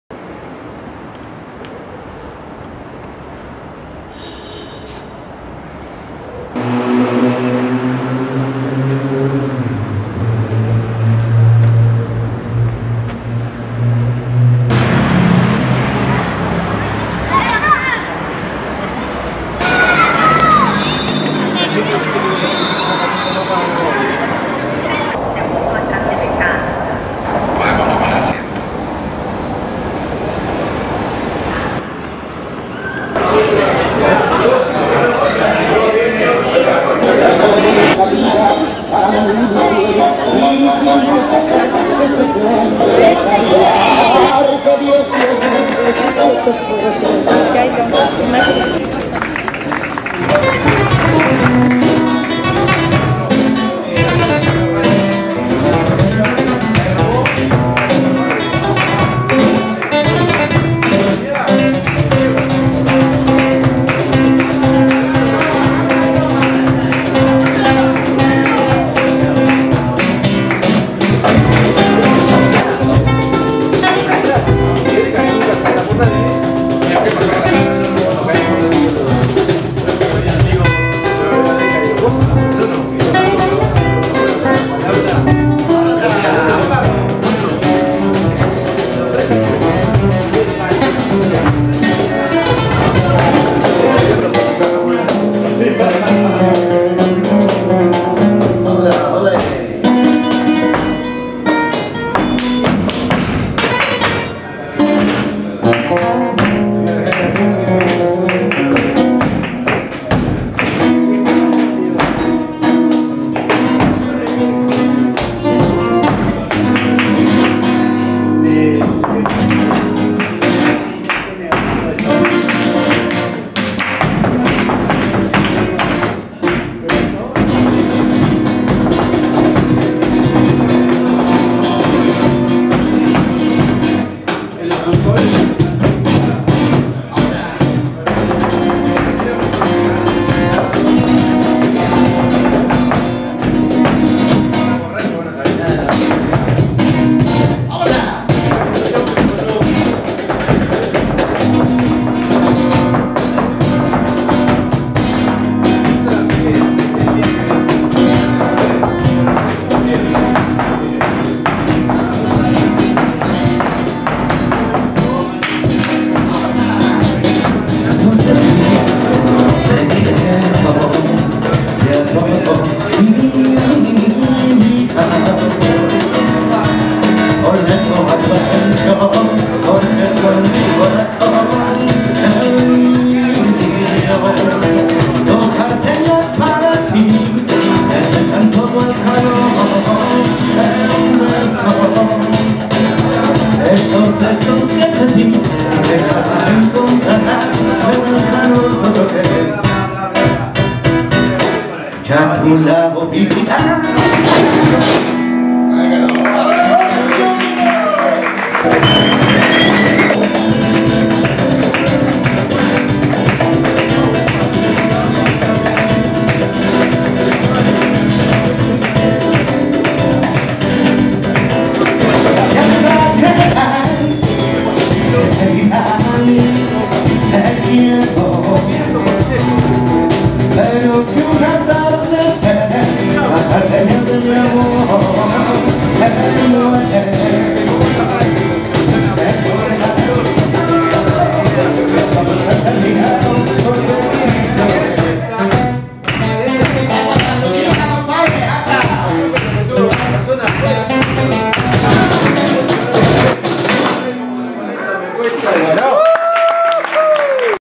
avond in Buenos Aires
Buenos Aires by night. In cafe Coyhneche - Martinez - flamengo performance by Maximiliano Anastasi a.o.! April 20, 2014